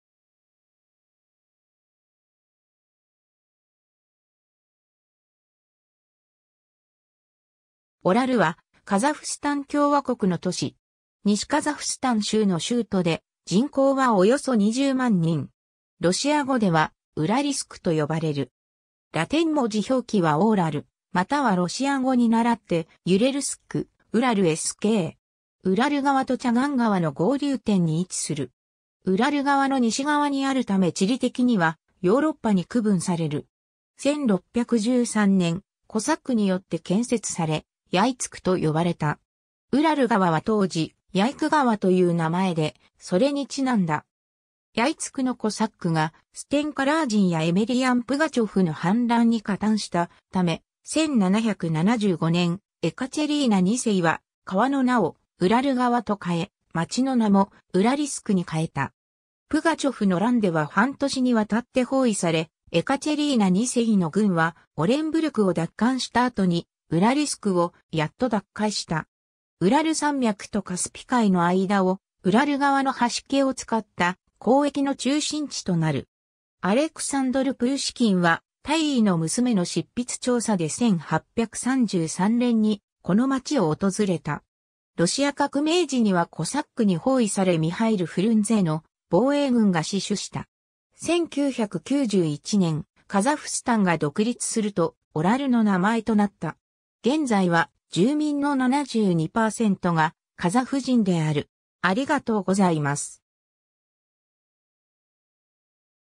オラル
オラル（カザフ語: Орал、英: Oral）は、カザフスタン共和国の都市。
西カザフスタン州の州都で、人口はおよそ20万人。
ロシア語ではウラリスク（Уральск）と呼ばれる。
ラテン文字表記はOral、またはロシア語に倣ってUralsk、Ural'sk。
 ウラル川とチャガン川(Chagan)の合流点に位置する。
ウラル川の西側にあるため地理的にはヨーロッパに区分される。
 1613年、コサックによって建設されヤイツク(Yaitsk)と呼ばれた。
ウラル川は当時、ヤイク川(Yaik)という名前で、それに因んだ。
ヤイツクのコサックが、ステンカ・ラージンやエメリヤン・プガチョフの反乱に加担したため、1775年、エカチェリーナ2世は川の名を「ウラル川」と変え、町の名も「ウラリスク」に変えた。
プガチョフの乱では半年にわたって包囲され、エカチェリーナ2世の軍はオレンブルクを奪還した後に、ウラリスクをやっと奪回した。
ウラル山脈とカスピ海の間を、ウラル川のはしけを使った交易の中心地となる。
アレクサンドル・プーシキンは「大尉の娘」の執筆調査で1833年にこの町を訪れた。
ロシア革命時にはコサックに包囲されミハイル・フルンゼの防衛軍が死守した。
1991年、カザフスタンが独立すると、オラルの名前となった。
現在は住民の72%がカザフ人である。